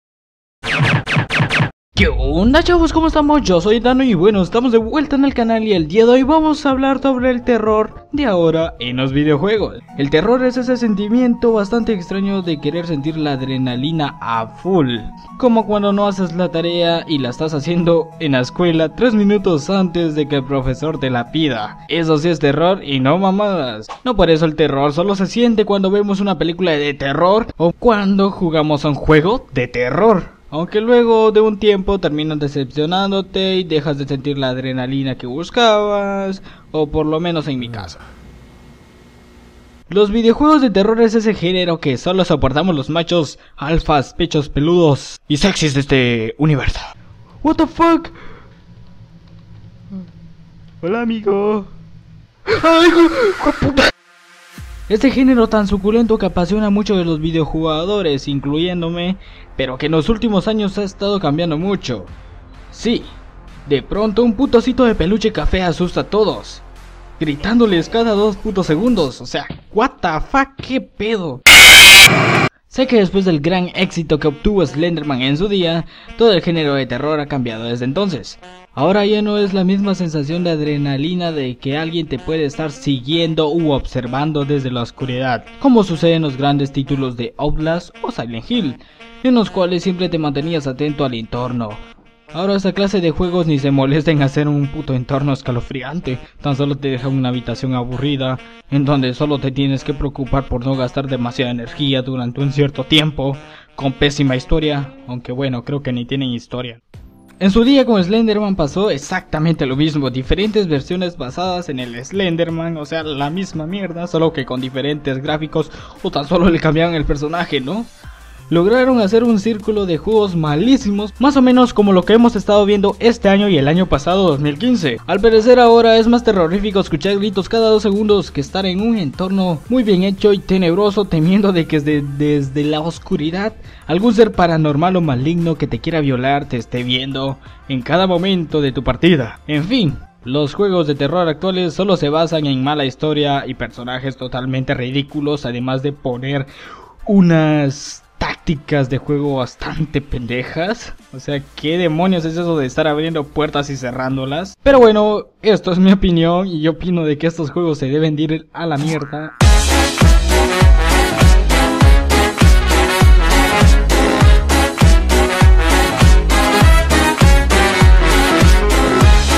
EL TERROR DE HOY EN LOS VIDEOJUEGOS
:3 Este vídeo me ha hecho sentir muy bien, ya tenia mucho tiempo queriendo hacer esto, nunca me gusto la sobre explotación que muchos youtubers le hicieron a este juego, del cual se aprovecharon a no mas poder, y las consecuencias fueron que salieran copias cada vez mas horribles y aburridas que el mismo juego en si.